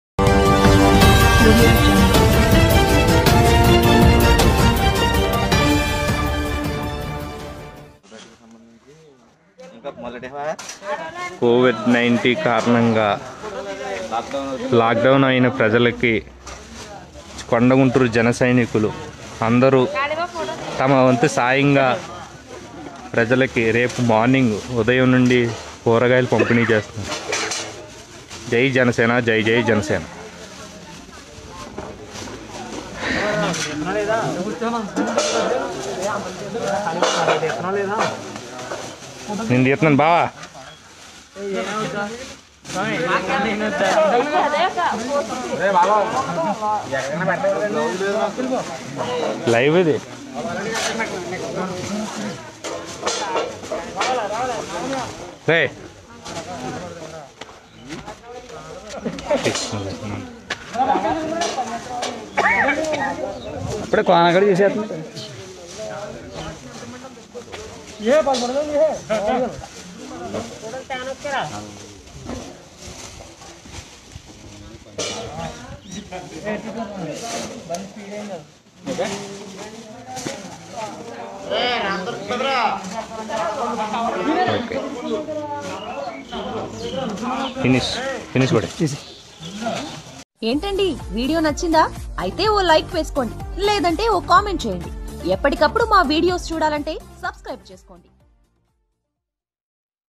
వేల మందికి కూరగాయలు పంపిణి కి సిద్ధం చేస్తున్న జనసైనికులు | Janasena Party | Pawan Kalyan | FFN
Watch#